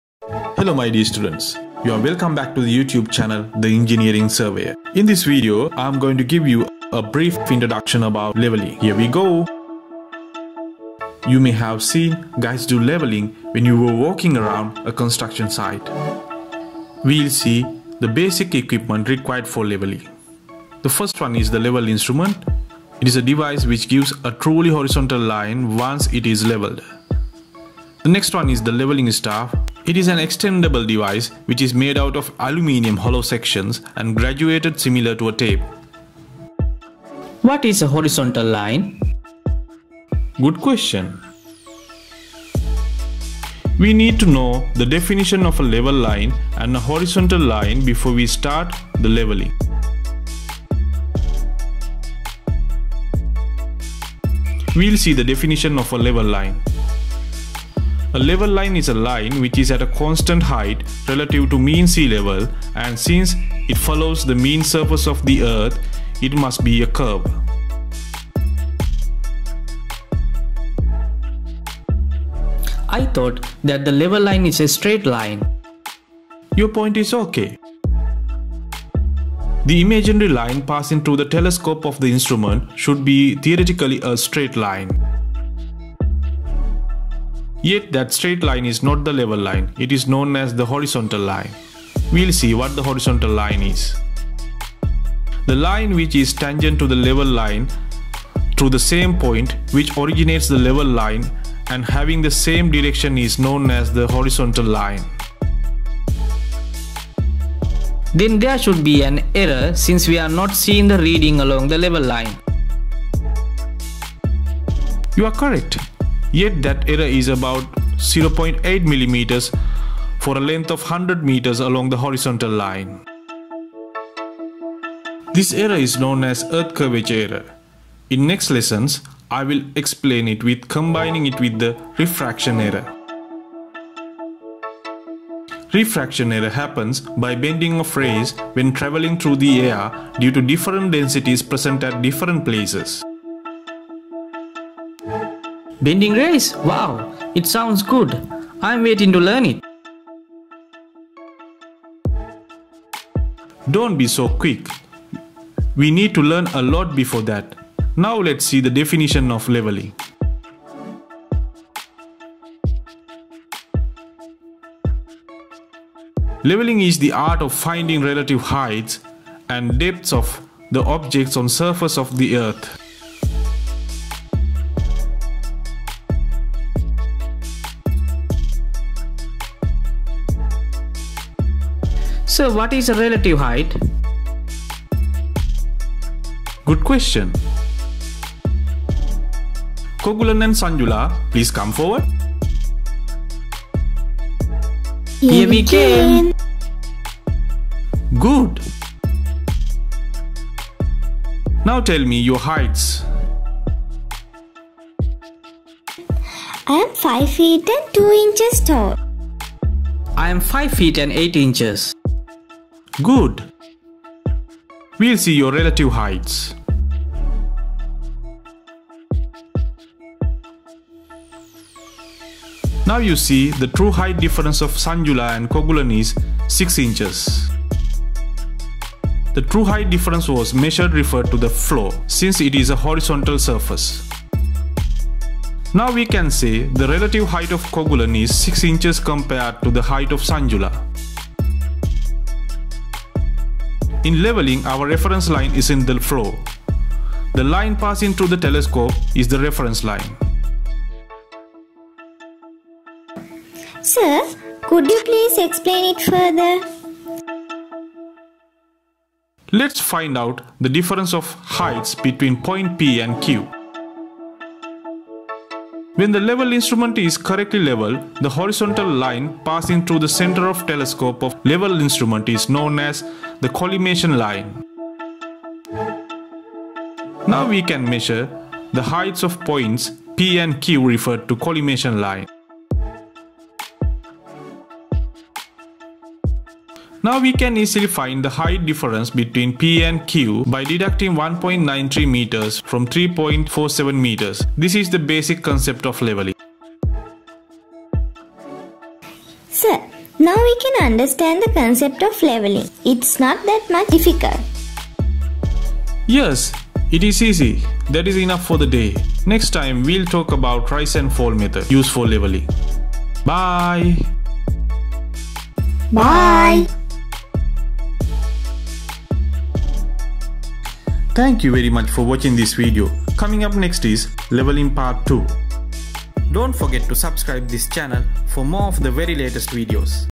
Introduction to leveling
This is made out to teach the students the basics of leveling. After watching this video, viewers will realize that leveling is not very much difficult as they thought.